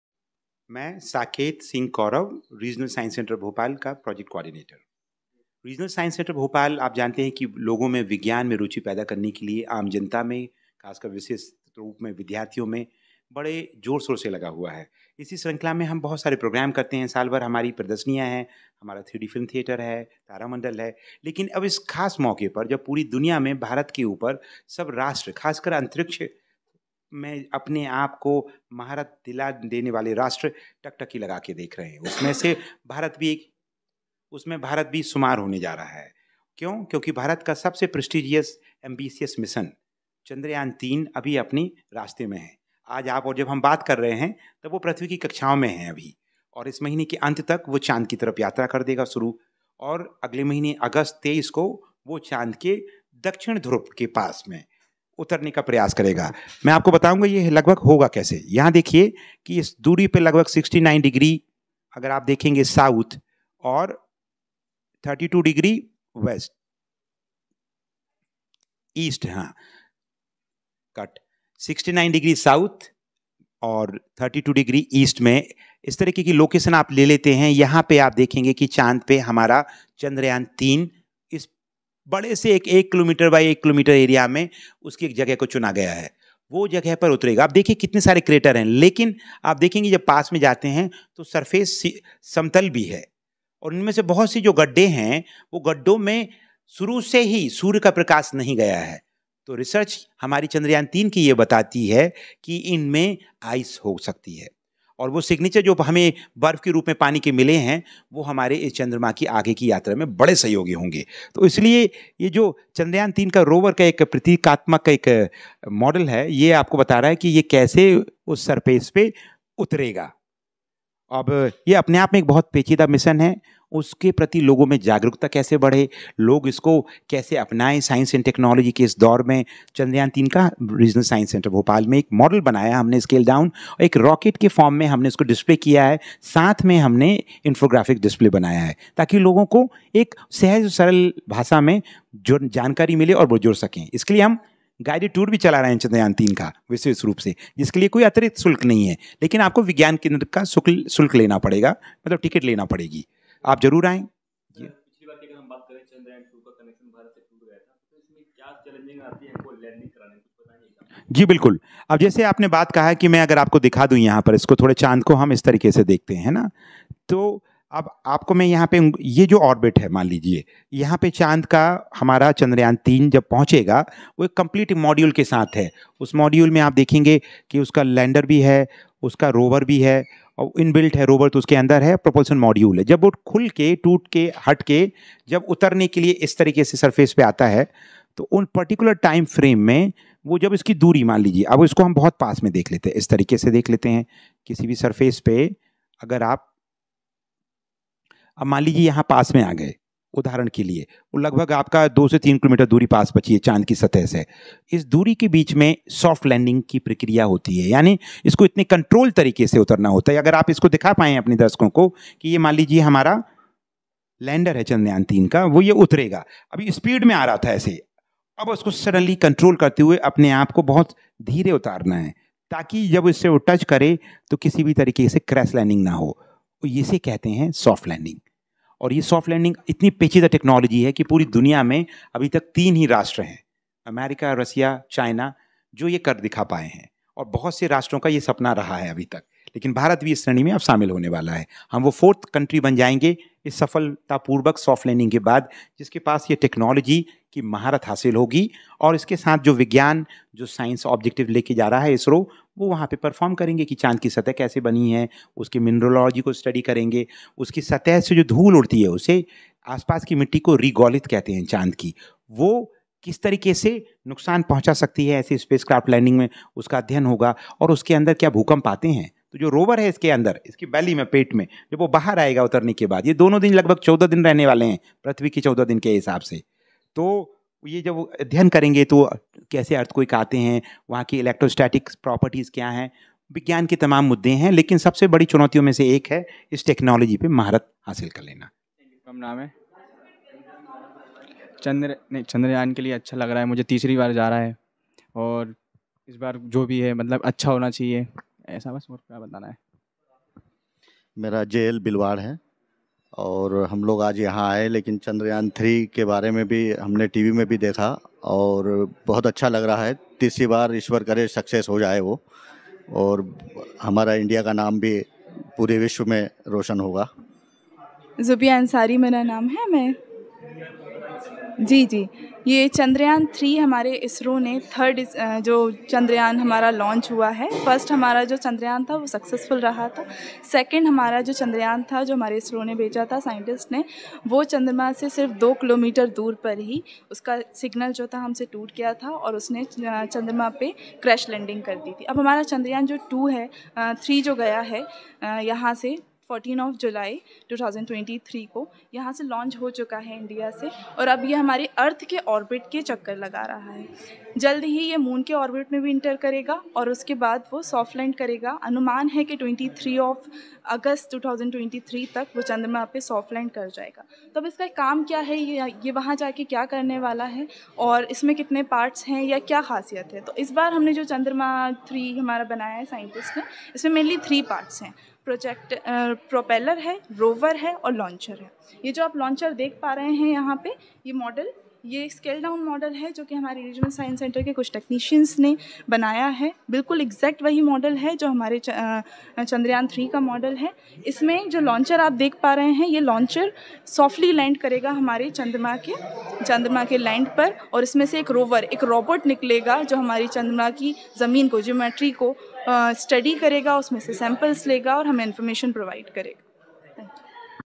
Chandrayaan-3 की सॉफ्ट लैंडिंग आज, देशभर में हो रही है पूजा-अर्चना
Bhopal News: 23 अगस्त 2023 भारत के लिए गर्व का दिन होने वाला है. आज भारत चंद्रमा पर लैंड करने वाला दुनिया का चौथा देश बनने जा रहा है. इस विशेष महत्व वाले दिन को सेलिब्रेट करने और चंद्रयान- 3 के रोवर को चांद पर लैंडिंग प्रक्रिया को आमजन को समझाने के लिए भोपाल के रीजनल साइंस सेंटर में एग्जीबिशन लगाया गया है.
Bhopal News: 23 August 2023 is going to be a day of pride for India. Today India is going to become the fourth country in the world to make land on the Moon. Ecolibration has been organized at the Regional Science Center in Bhopal to celebrate this special day and to include the launch of Chandrayaan-3 rover on the Moon.